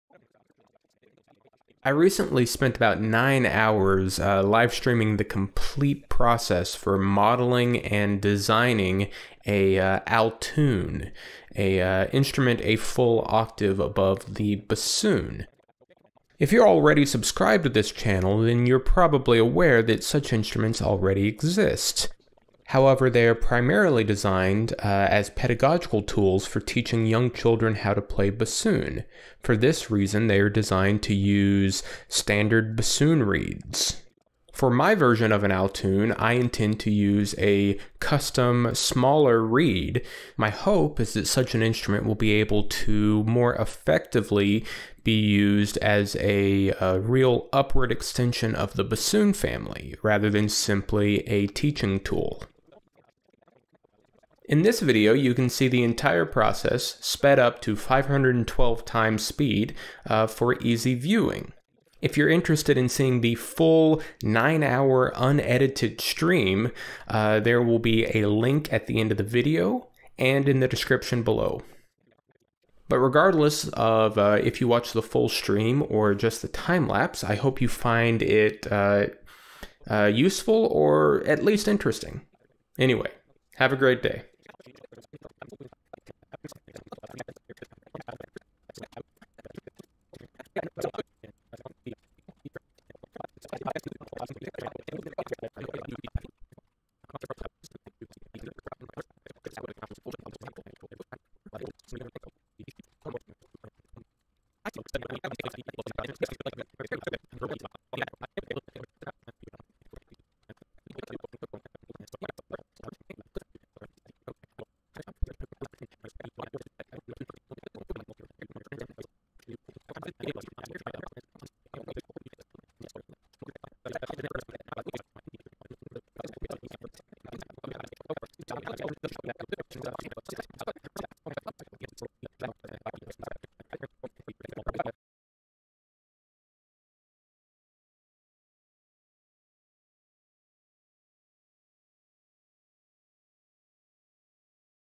Altoon: Designing and Modeling Timelapse
A timelapse of the 9-hour process of designing and modeling a prototype for my version of an altoon.  If you're interested in watching the unedited streams, you can find them in the unlisted playlist linked below: